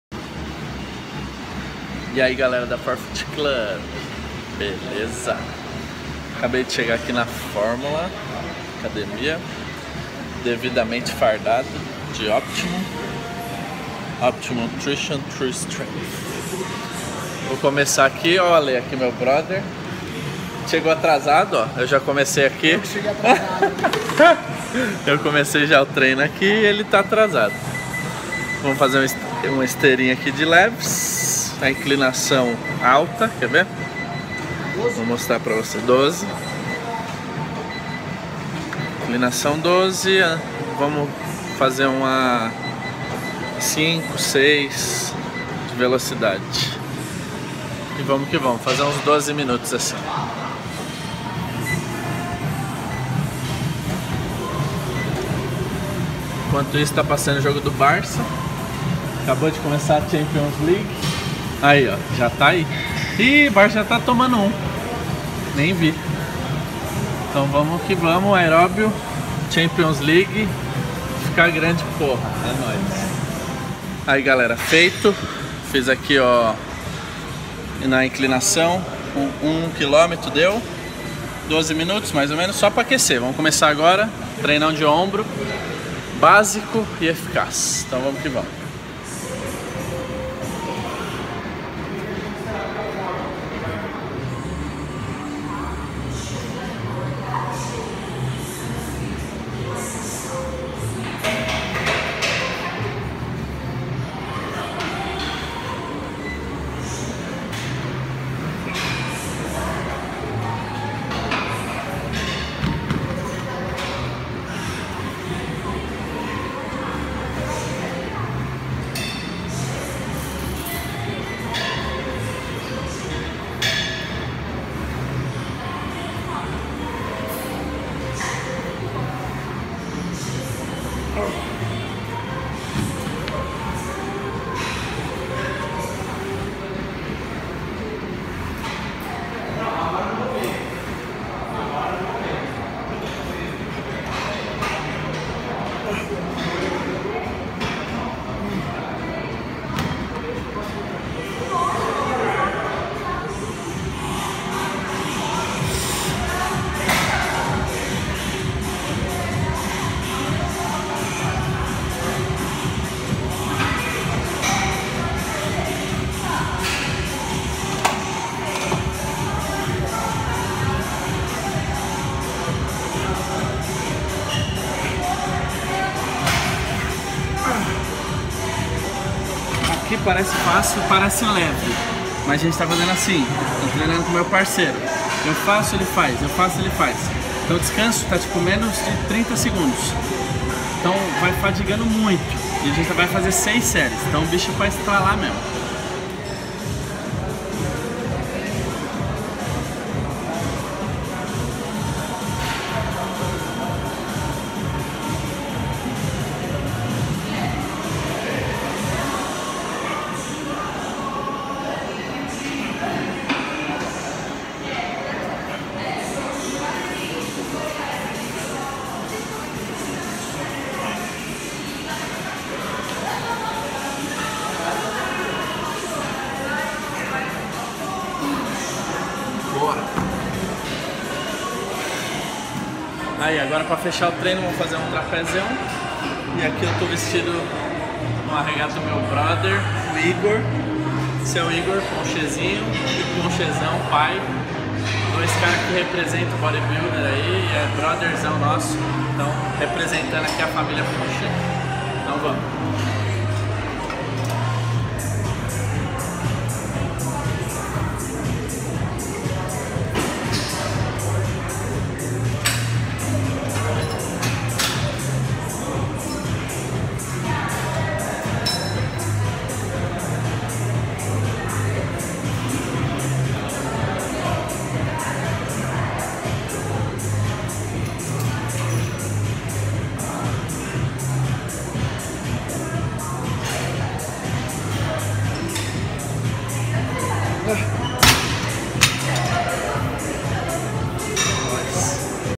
Treino de Ombros FIKAGRANDEPORRA 2
Treino de ombros do Renan Corrêa. O treino de ombros do Renan dessa vez foi um treino tradicional, exercícios básicos e movimentos básicos. Como o Renan voltou agora do EUA, é apenas um treino para voltar à rotina forte. Vamo que vamo FIKAGRANDEPORRA!